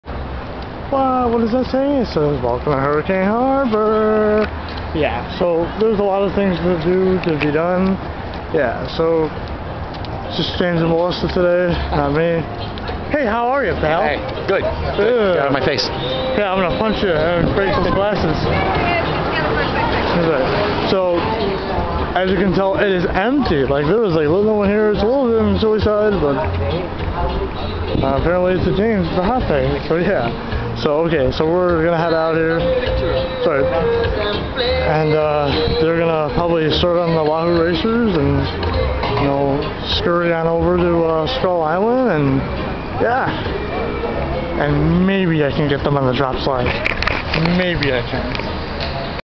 Welcome To Hurricane Harobr At Six Flags Great America
Welcome to Hurricane Harbor folks! Here we go, Lets see what JiggidyNDG and Jacklover19 can cover here.
Shot on 7/24/13 with Kodak Easyshare C182